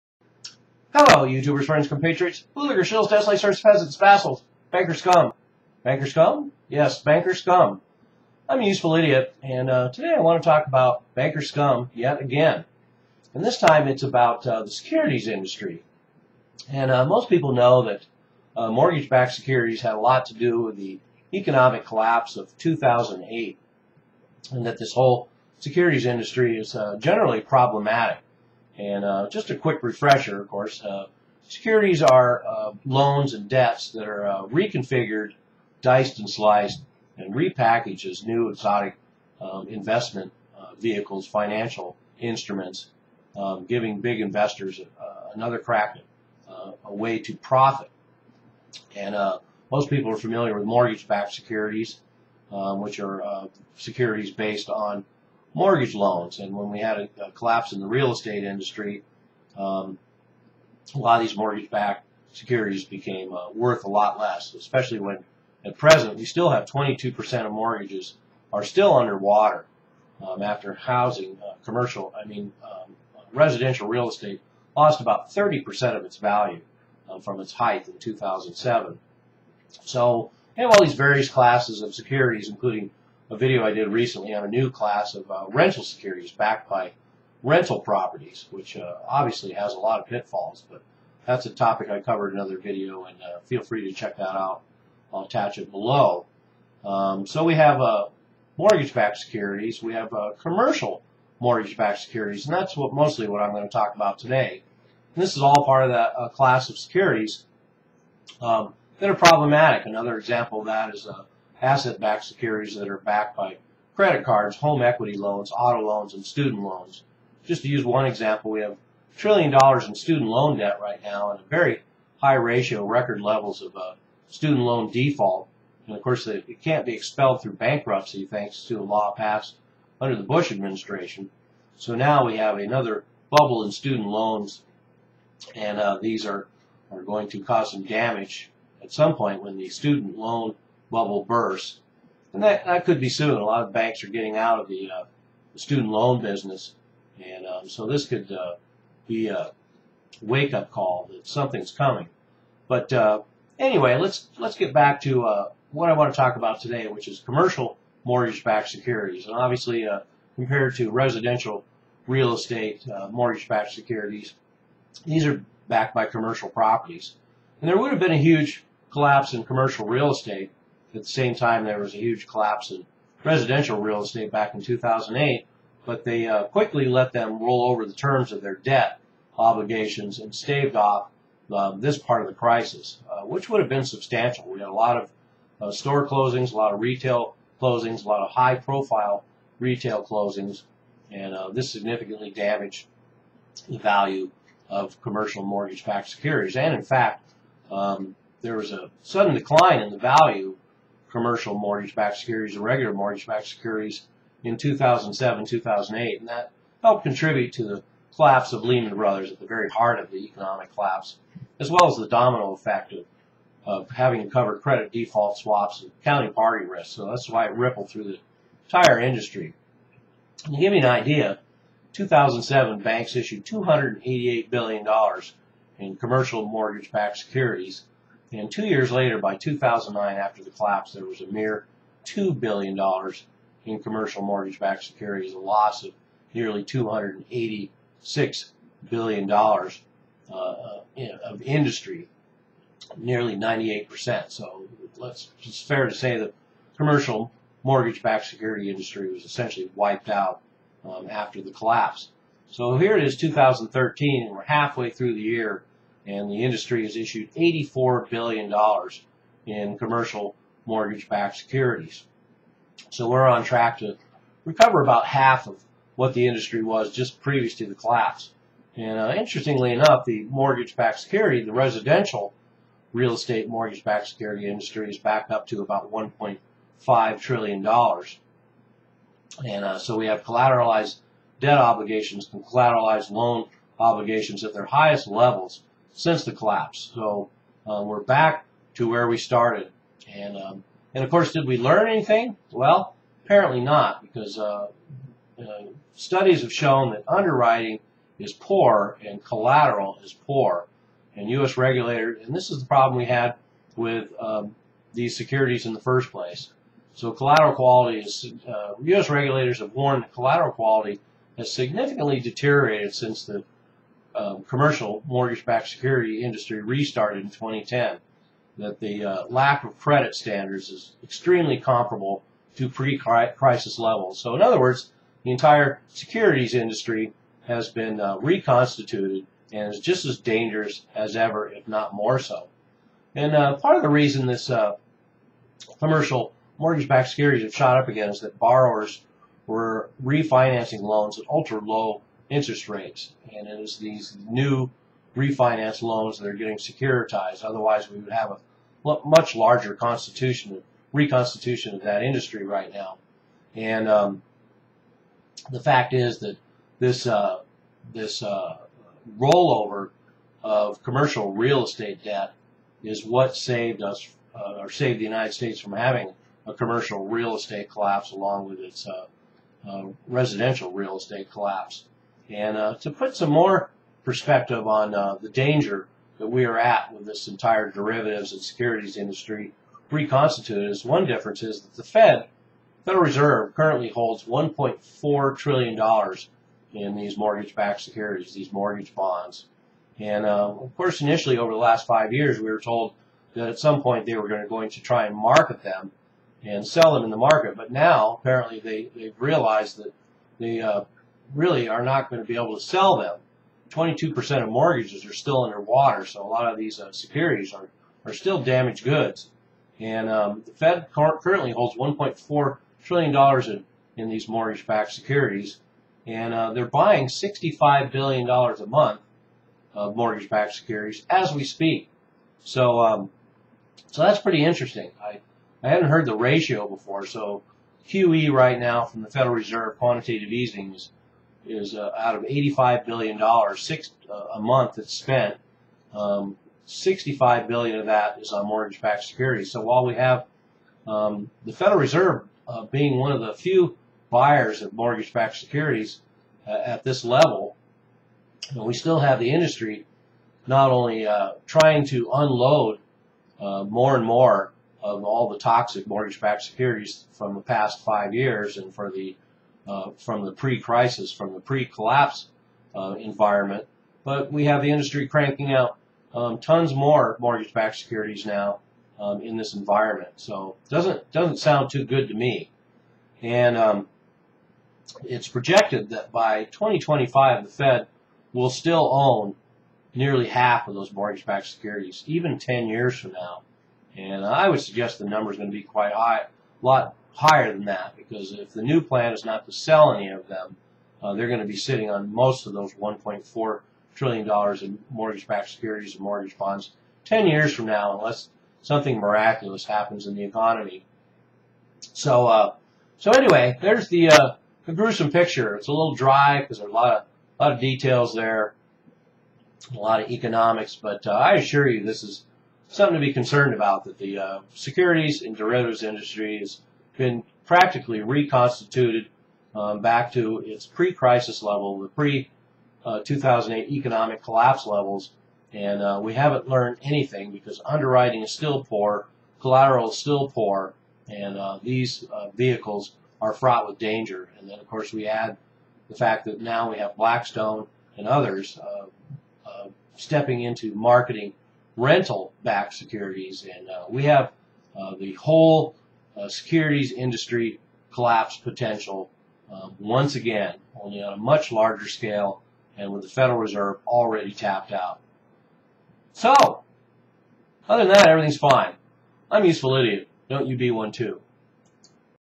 Banks Rebuild Dangerous Securities Industry
A Useful Idiot discusses the news that the Commercial Mortgage Backed Security (CMBS)  and Residential Mortgage Backed Security (MBS) industries have been rebuilt to pre-economic collapse levels...only this time there is still 22% of mortgages underwater, the Federal Reserve has  $1.4 trillion of MBS on their balance sheet and is still buying them to the tune of $65 billion a month...And, just like last time, there is questionable underwriting,credit standards, and collateral. Add to this the continued collapse in the retail sector adding to the decay of CMBS, the student loan debt bubble along with associated ABSs, and the new subprime auto loans also turned into ABSs,the new "Rental Backed Securities", as well as all the connected counterparty risk and associated Credit Default Swaps...and what could possibly go wrong?